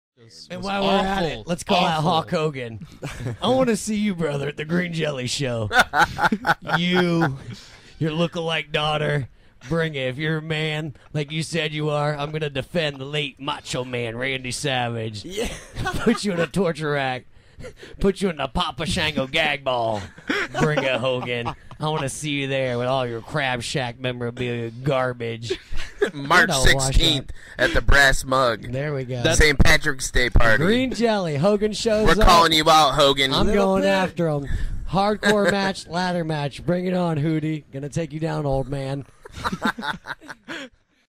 Mosquito Teeth calls out Hulk Hogan!
See you there. if you're man enough!

3/16/13 - Brass Mug, Tampa, FL